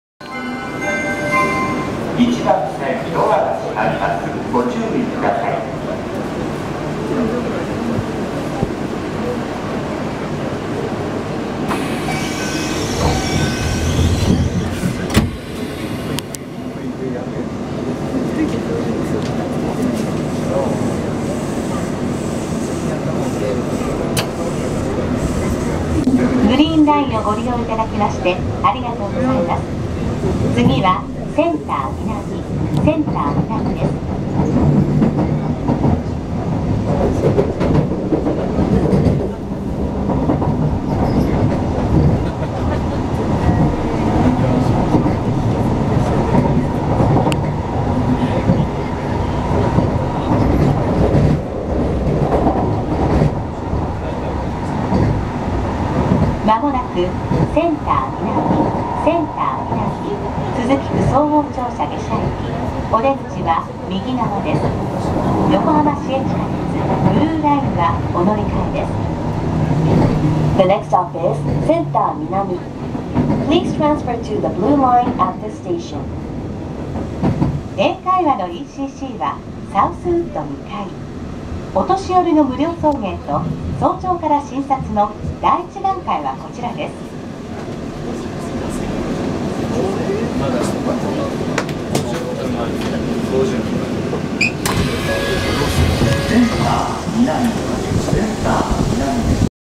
横浜市営地下鉄グリーンライン普通中山行き10000形側面展望 センター北→センター南
やっぱりリニア地下鉄は速いですね。